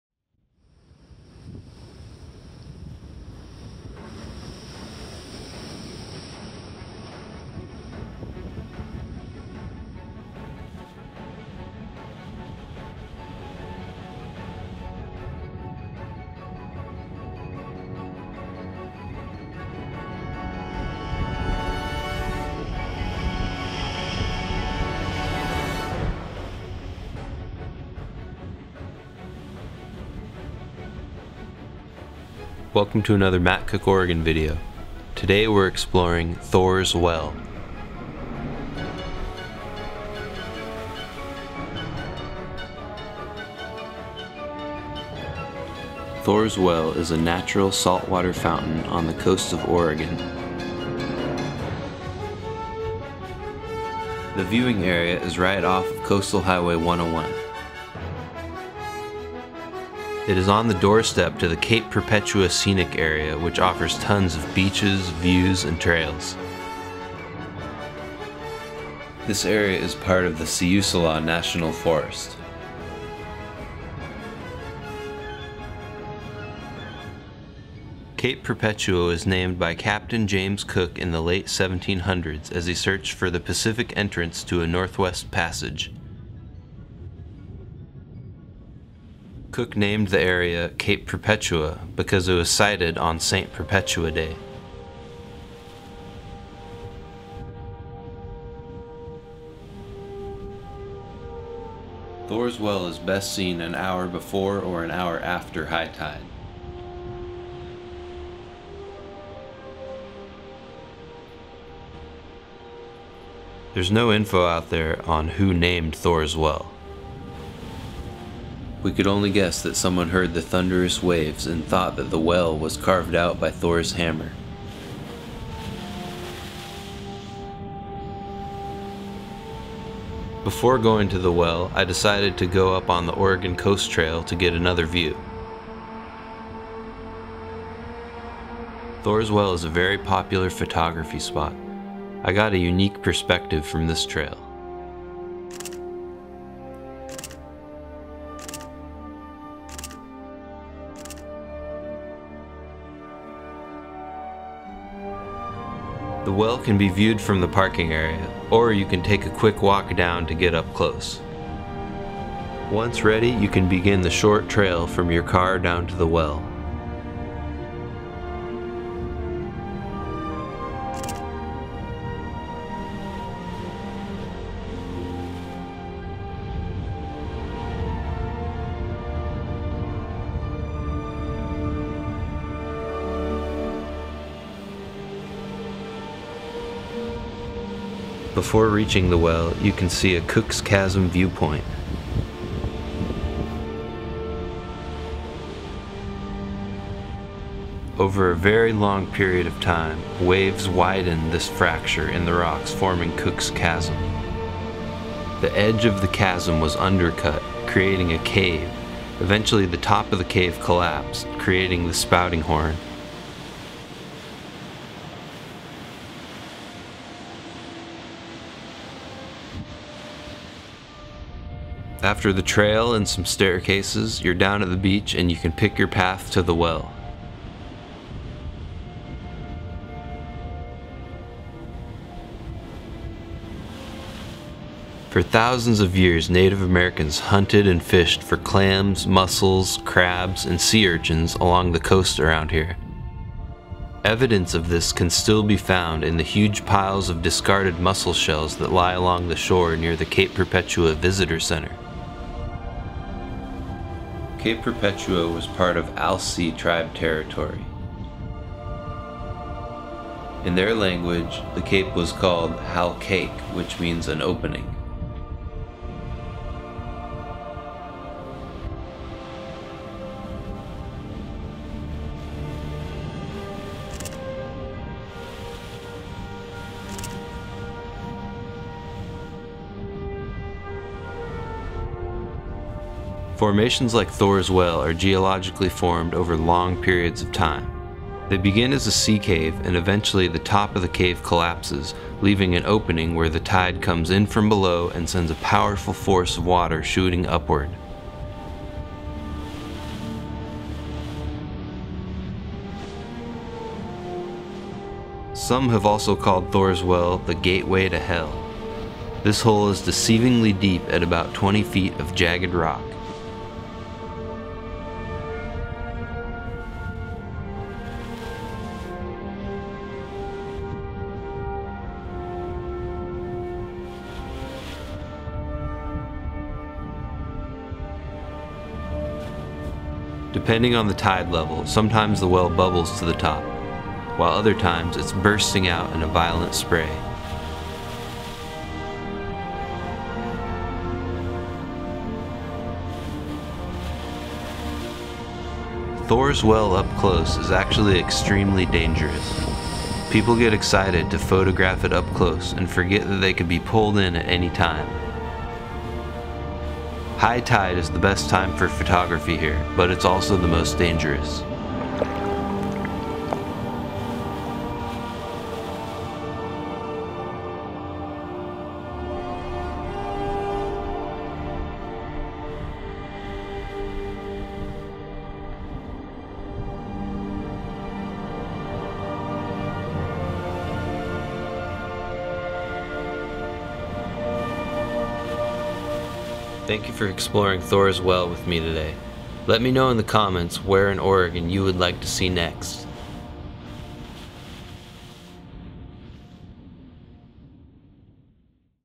Thor's Well: Amazing natural wonder at Cape Perpetua, Oregon
In this episode we explore Thor's Well. This is a beautiful natural salt water fountain on the Oregon coast. This spot is very popular for photography because of its unique features. The recording was done on a few different days, so you will see different tide levels and weather. This spot can be deceivingly dangerous so be careful.

Support quality free and educational media from the outdoors of Oregon.